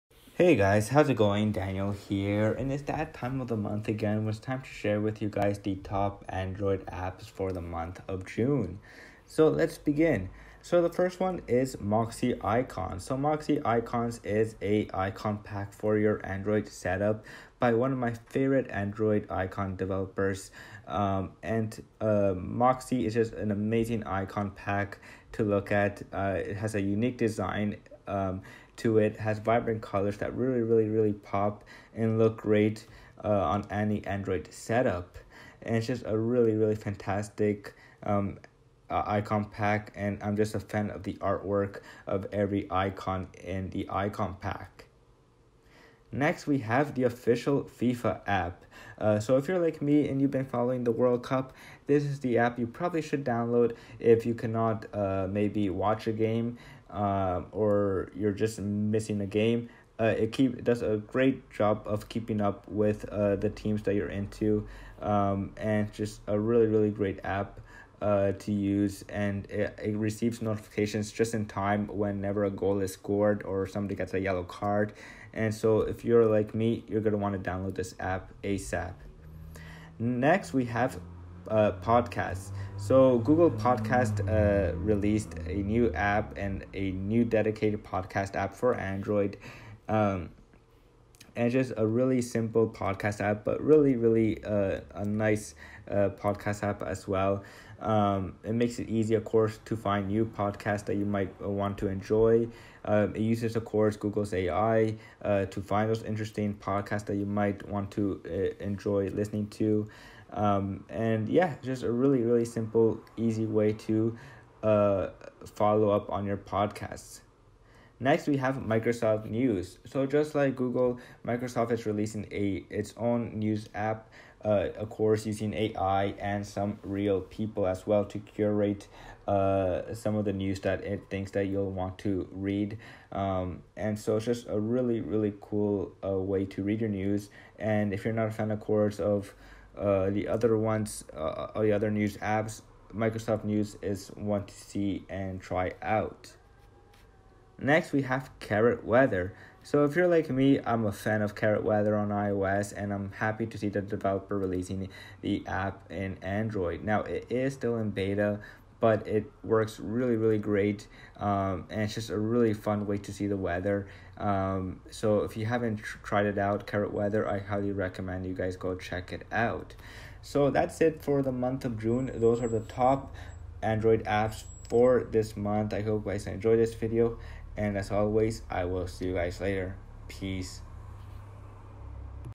Top Android Apps For June 2018
These are the top Android apps for the month of June 2018.

Apps
Moxy Icons
FIFA App
Google Podcast
Microsoft News
Carrot Weather

 ***My Gear***
Canon T3
iPhone SE
Nexus 6